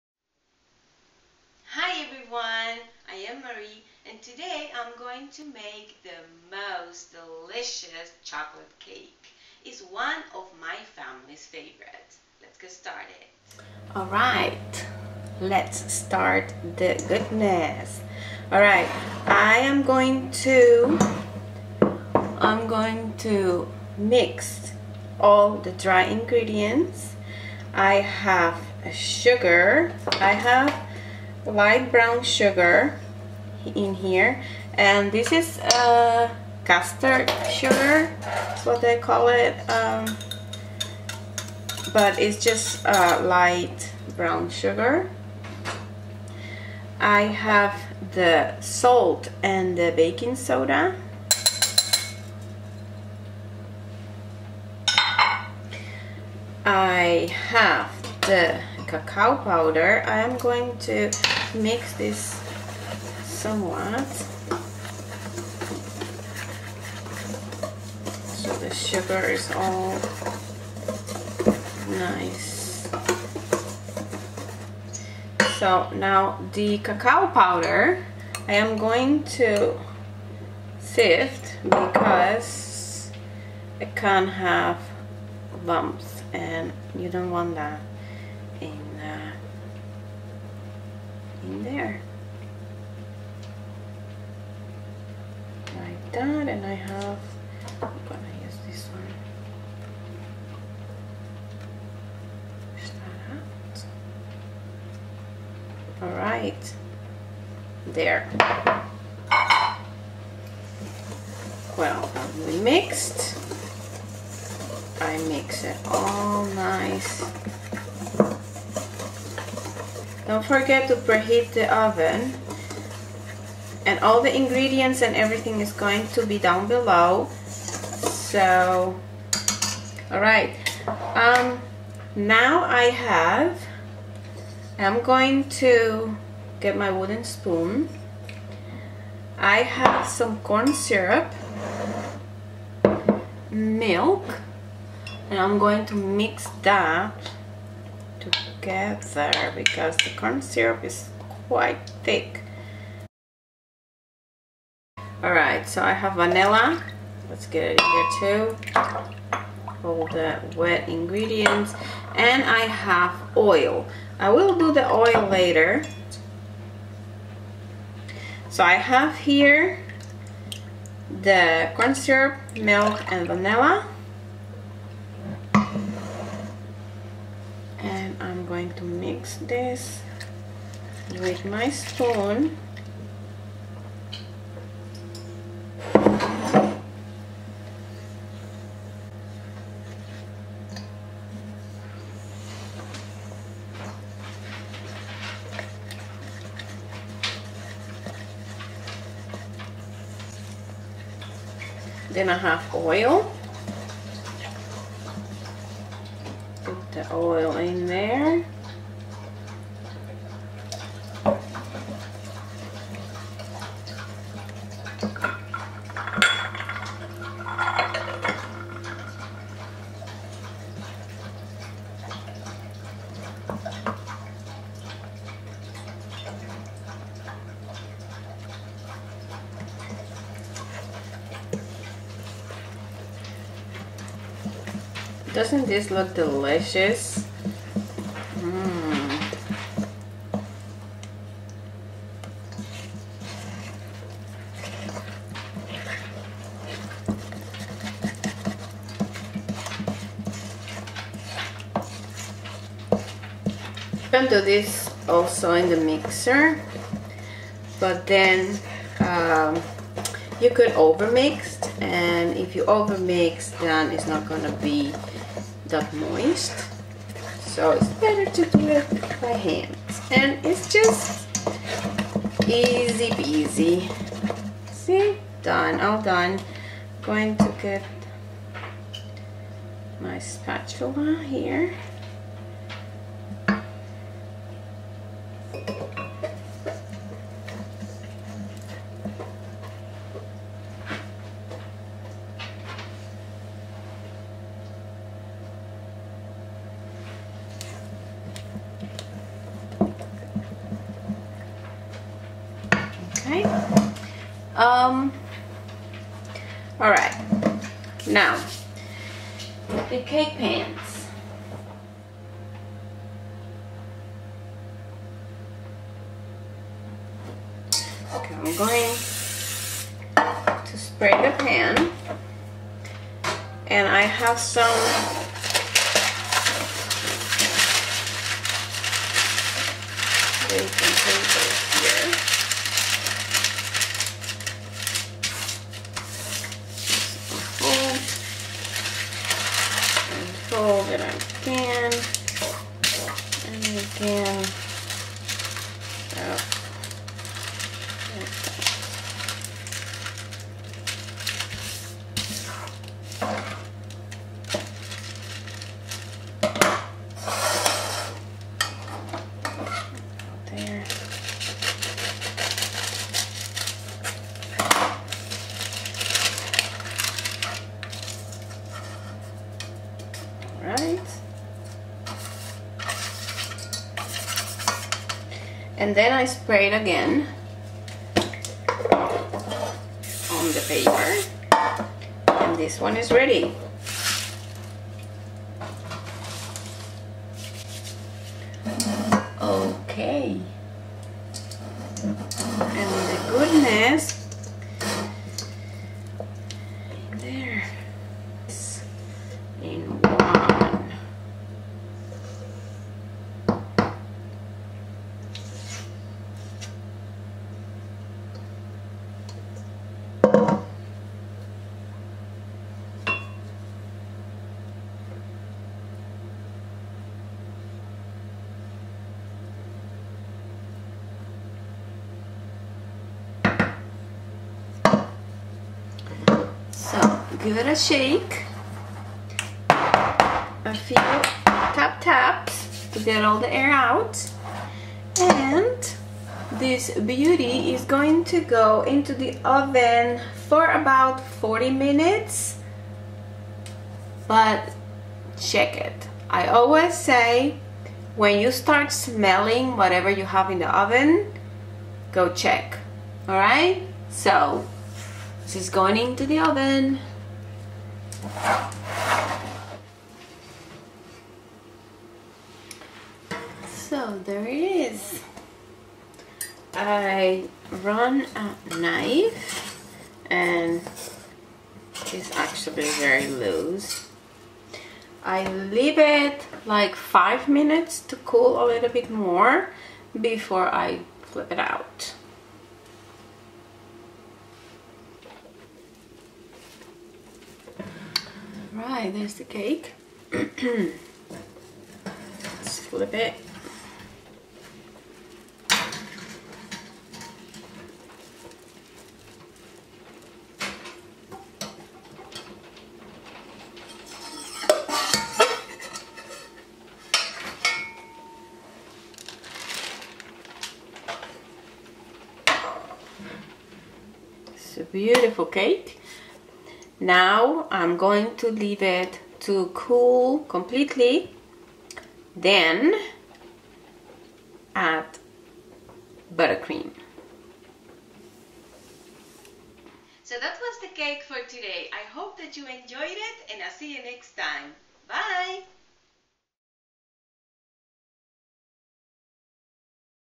Chocolate Cake (Baking weekend 1a)
Hi Everyone here the most delicious chocolate cake ,hope you enjoy xx

Chocolate cake :
you will need
275g self rising flower
1/2tsp salt
3 tbs cacao powder
175 caster sugar I used brown
1 tsp baking soda
300ml milk
150ml oil
3tbs golden syrup
1tbs vanilla extract
pre heat oven 180c /355f
bake for about 40 minutes